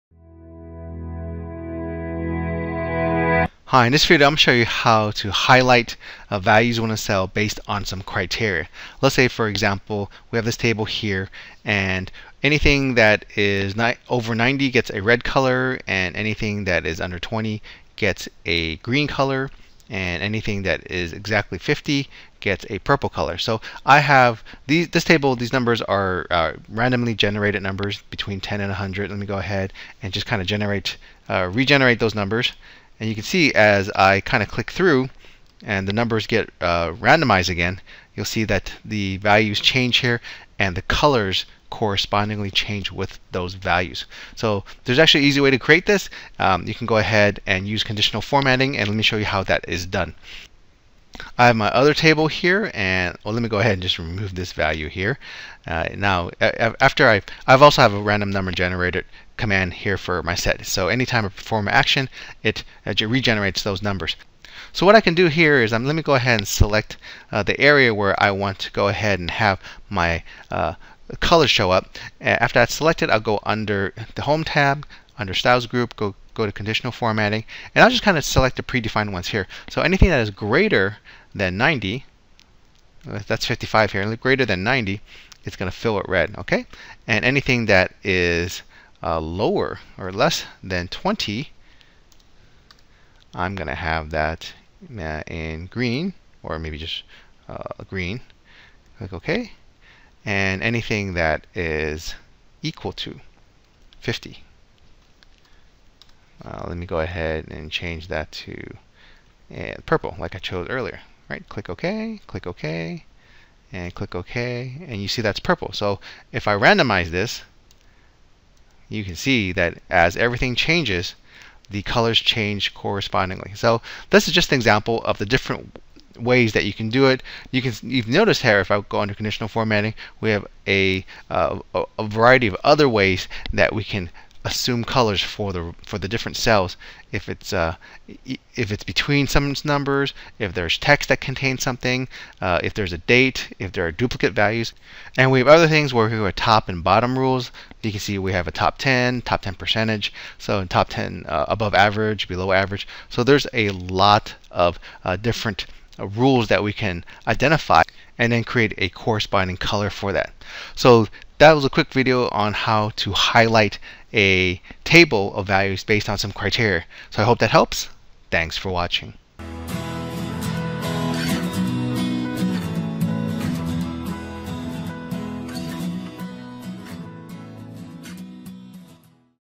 Highlight Cells That Meet Some Criteria
Excel is great for data analysis but sometimes it's hard to see the important data in a sea of letters or numbers.  That's why having the ability to highlight or show data with conditional formats are so useful. This video shows how to use conditional formatting to highlight values in a table based on criteria.